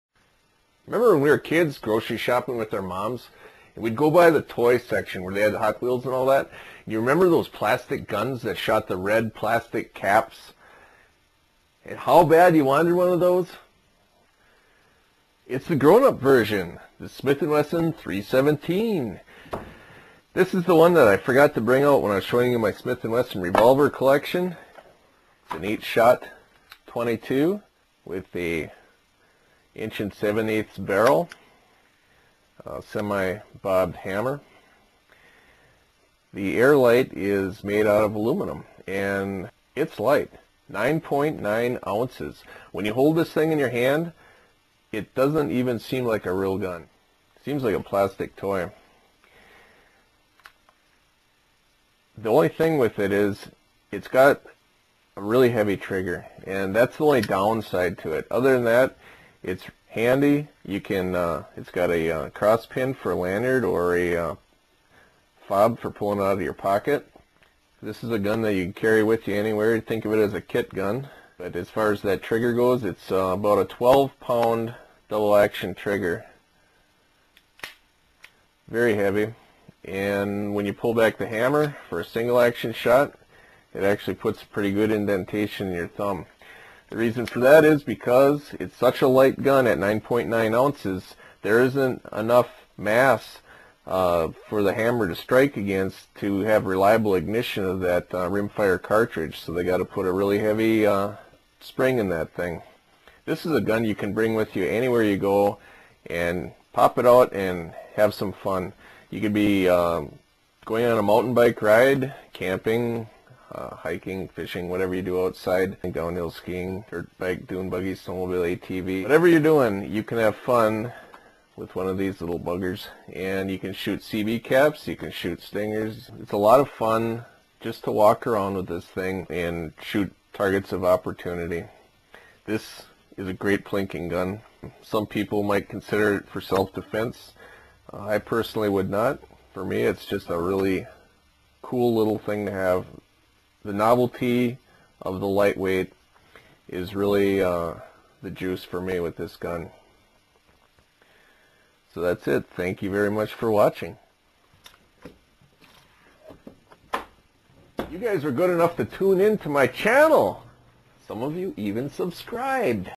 Smith & Wesson 317 AirLite Revolver
Here's the gun I forgot to include in my S&W Revolver Collection video.